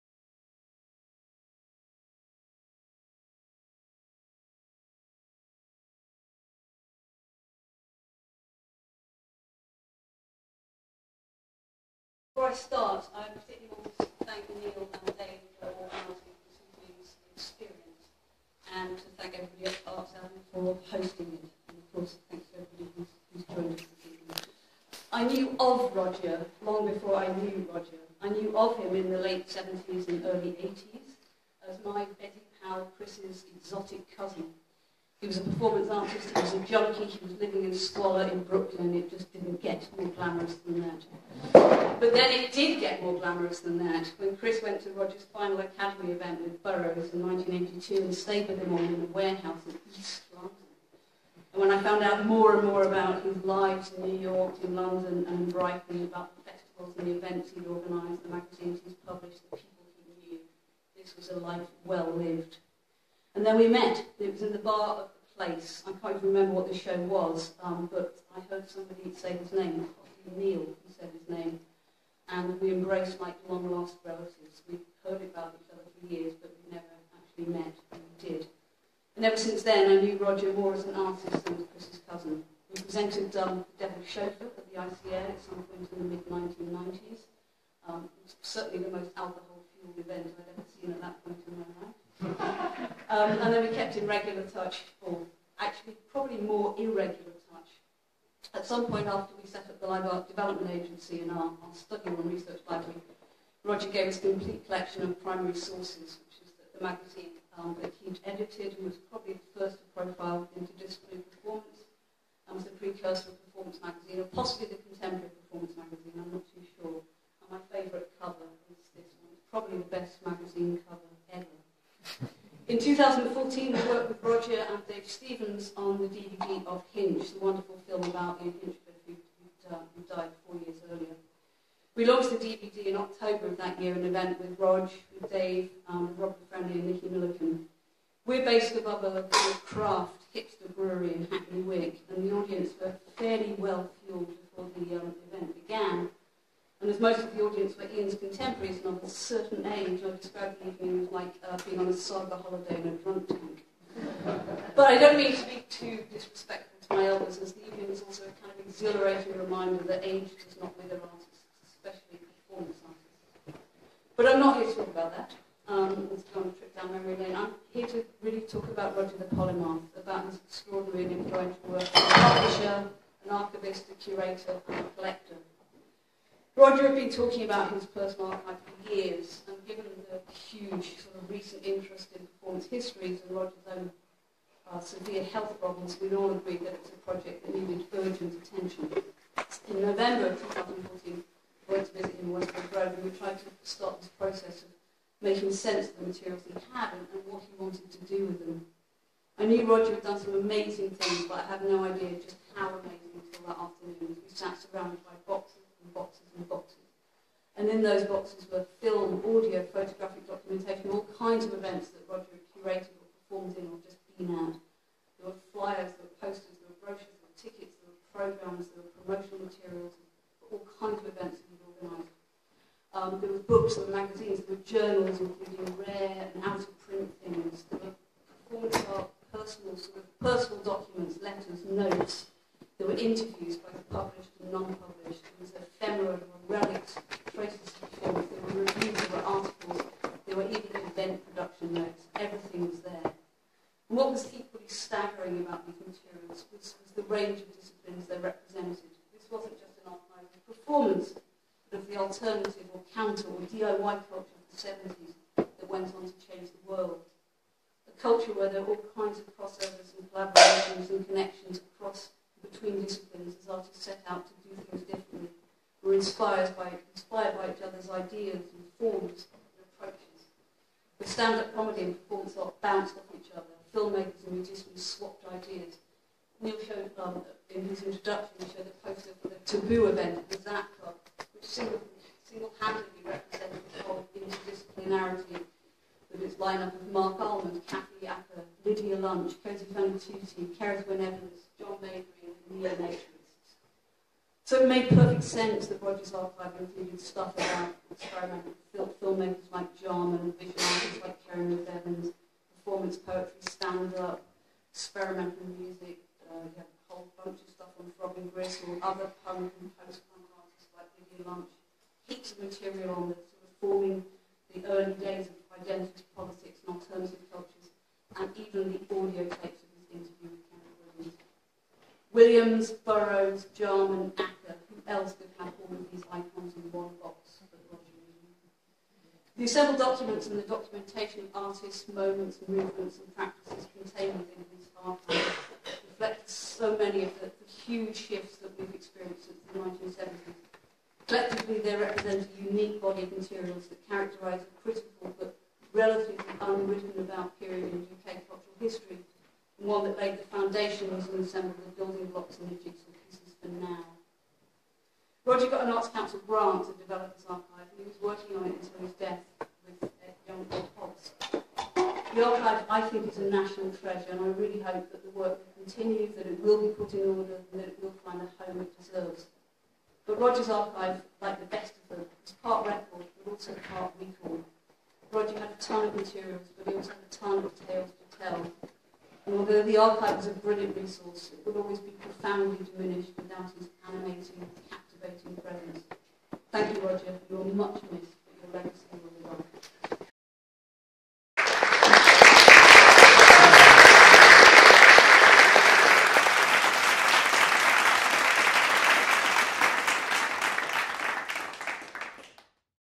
Lois Keidan Thank You Roger Ely
Lois Keidan pays tribute to the artist and curator Roger Ely.